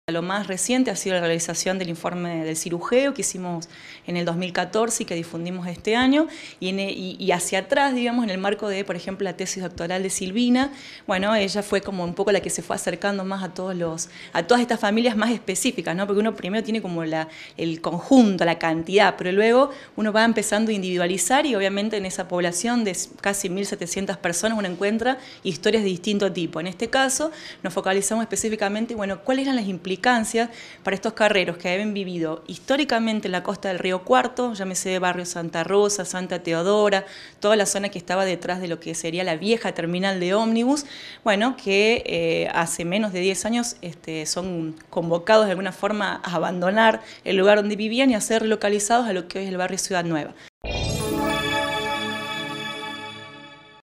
Carreros relocalizados: Claudia Kenbel
Claudia Kenbel, del Departamento de Ciencias de la Comunicación, comentó sobre la proyección de dicho documental, en Letras con Café, a las 19hs.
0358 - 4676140